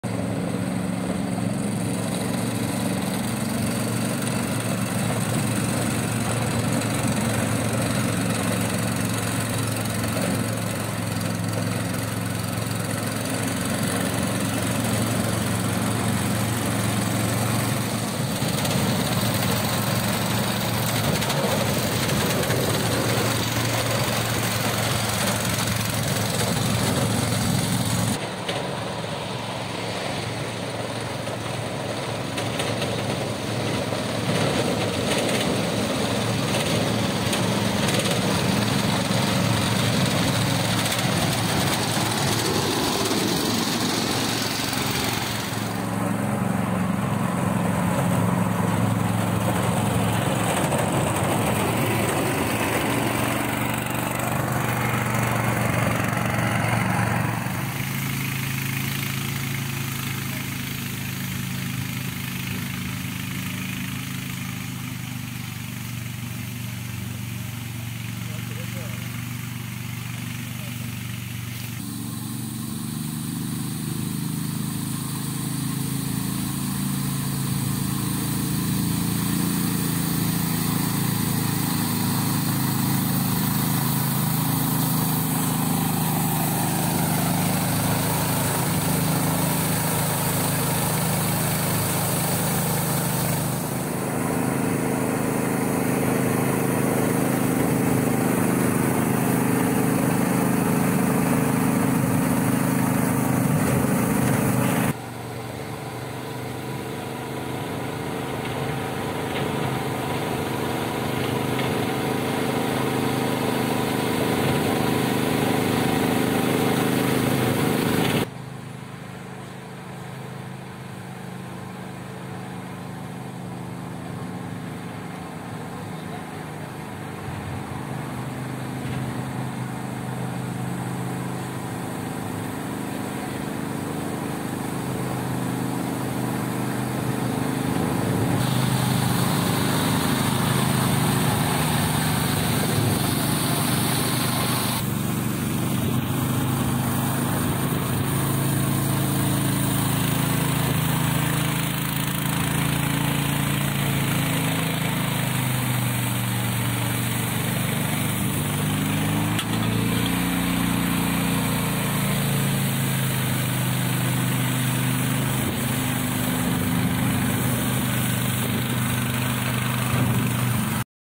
Remote Control Orchard Sprayer For Agriculture RC Robot Pesticide Spraying Machine
Long-term testing is an important method to verify the quality of the machine. We are constantly testing to prove the quality of our products.
Today we continue to test the remote control orchard sprayer near our factory.
We are a direct remote control sprayer and radio controlled lawn mower manufacturer from China.